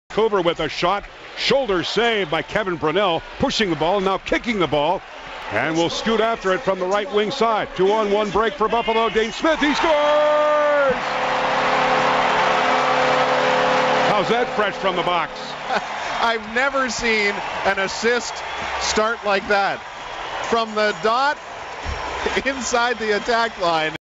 NLL: Kevin Brownell kicks pass and feeds Dhane Smith for Buffalo Bandits goal
Official YouTube of the NLL. to learn more about the National Lacrosse League.